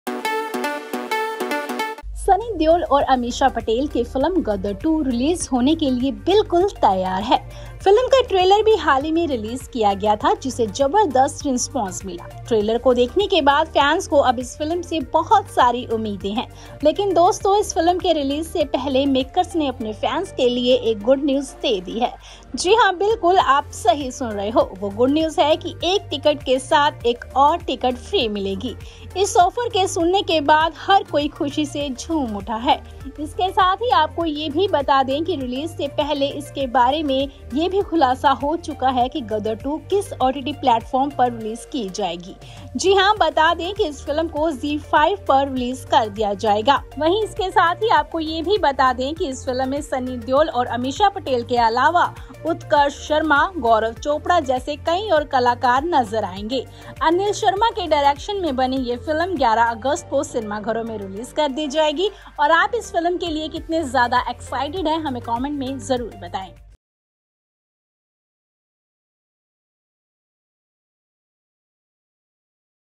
Gadar 2 Makers Surprises Audiences With ‘Buy One Get One’ Offer On Movie Tickets - 5 Dariya News
Gadar 2 Makers Surprises Audiences With ‘Buy One Get One’ Offer On Movie Tickets